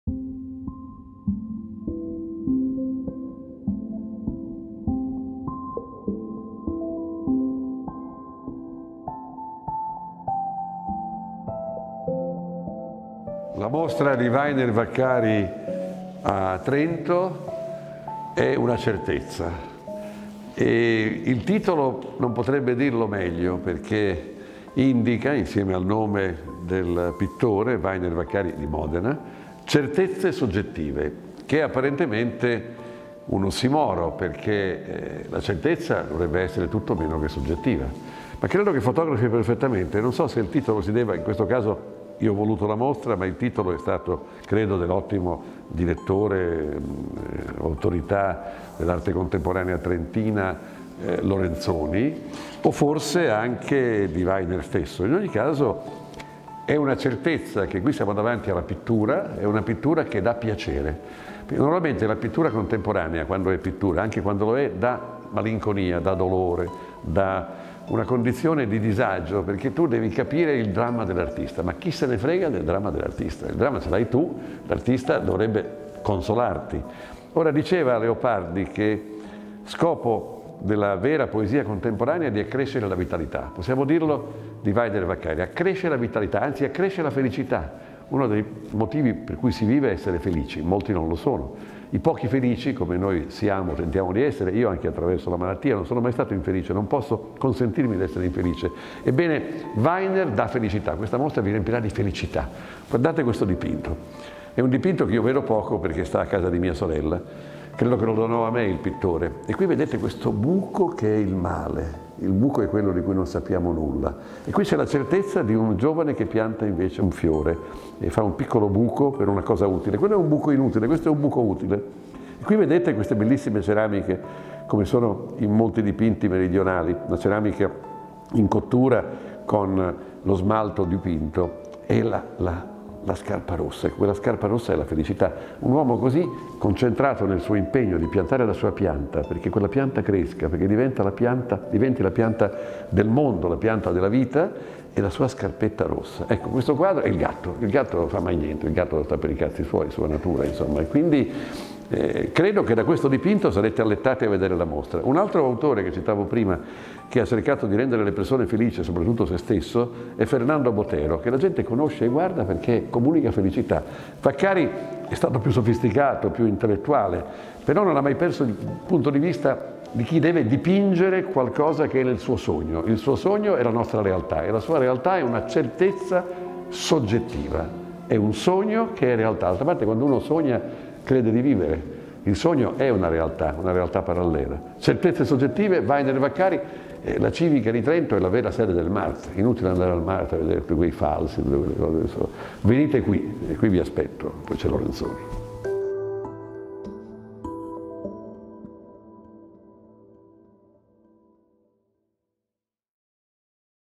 Pillole d'arte. Vittorio Sgarbi presenta la mostra "Weiner Vaccari. Certezze soggettive"
Il Presidente Vittorio Sgarbi presenta la mostra su Weiner Vaccari, che presenta un significativo spaccato della produzione di Vaccari: gli esordi, negli anni Ottanta e Novanta, e le opere più recenti attraverso lavori inediti provenienti da collezioni pubbliche e private. Alla Galleria Civica di Trento fino al 27 febbraio.

📍 Wainer Vaccari. Certezze soggettive
Da un’idea di Vittorio Sgarbi
A cura di Gabriele Lorenzoni
🎥 Mart, Giulia Lenzi